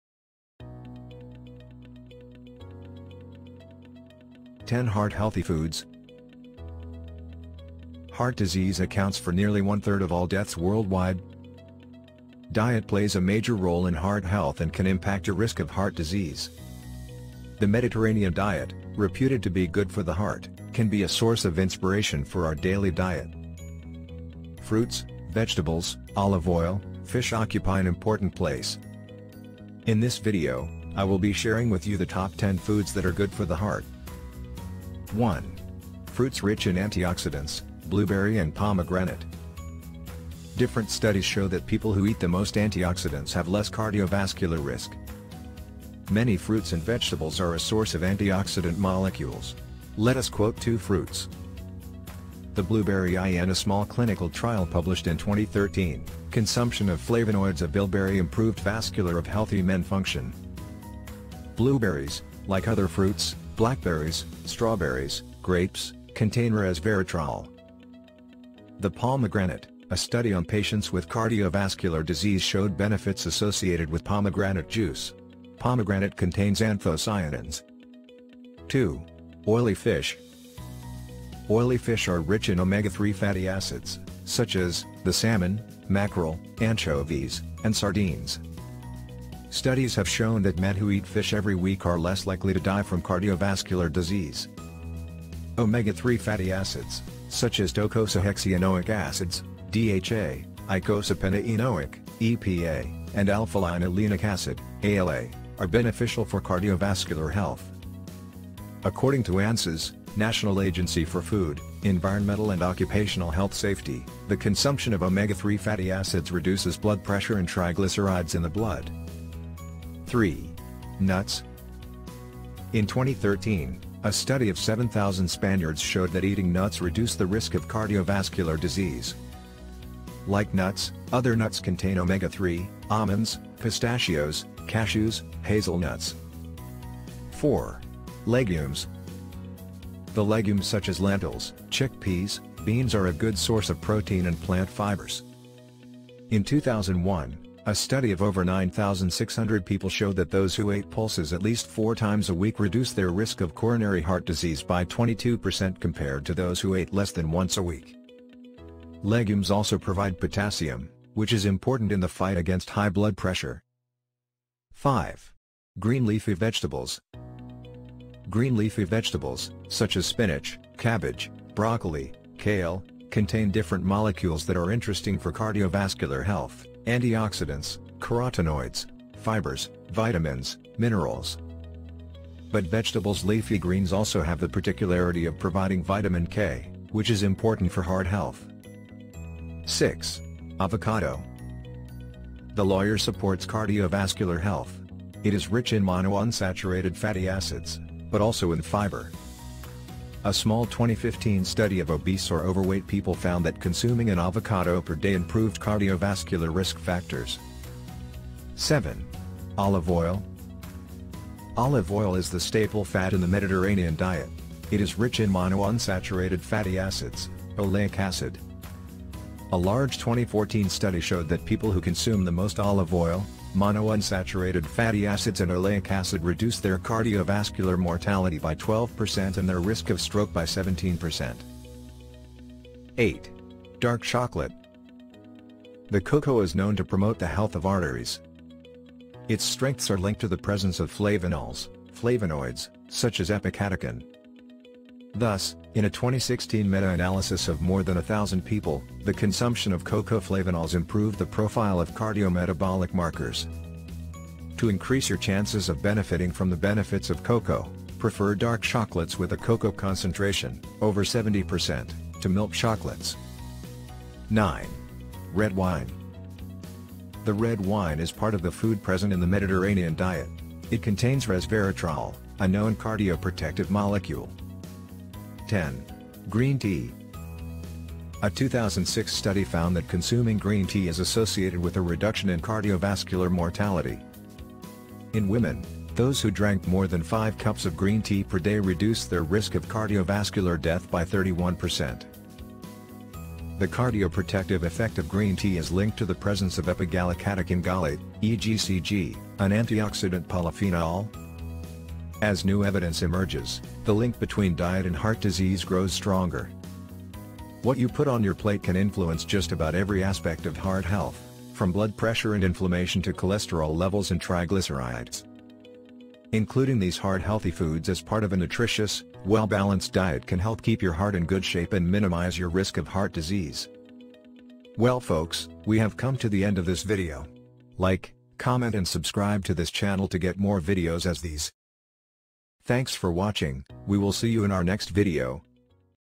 10 BEST FOODS THAT ARE GOOD FOR THE HEART | THE HEART HEALTHY FOODS
10 HEART-HEALTHY FOODS

The Mediterranean diet , reputed to be good for the heart, can be a source of inspiration for our daily diet: fruits, vegetables, olive oil, fish occupy an important place.

In this video, I will be sharing with you the top ten foods that are good for the heart.

1. Fruits rich in antioxidants: blueberry and pomegranate

Different studies show that people who eat the most antioxidants have less cardiovascular risk. Many fruits and vegetables are a source of antioxidant molecules. Let us quote two fruits:

The blueberry: in a small clinical trial published in 2013 , consumption of flavonoids of bilberry improved vascular of healthy men function. Blueberries, like other fruits (blackberries, strawberries , grapes ), contain resveratrol;

the pomegranate : a study on patients with cardiovascular disease showed benefits associated with pomegranate juice. Pomegranate contains anthocyanins.

2. Oily fish

Oily fish are rich in omega-3 fatty acids, such as: the salmon, mackerel, anchovies, and sardines.

Studies have shown that men who eat fish every week are less likely to die from cardiovascular disease.

Omega-3 fatty acids, such as docosahexaenoic acids (DHA), eicosapentaenoic (EPA), and alpha-linolenic acid (ALA), are beneficial for cardiovascular health. According to ANSES  (National Agency for Food, Environmental and Occupational Health Safety), the consumption of omega-3 fatty acids reduces blood pressure and triglycerides in the blood.

3. Nuts

In 2013, a study of 7,000 Spaniards showed that eating nuts reduced the risk of cardiovascular disease. Like nuts, other nuts contain omega-3: almonds, pistachios, cashews, hazelnuts.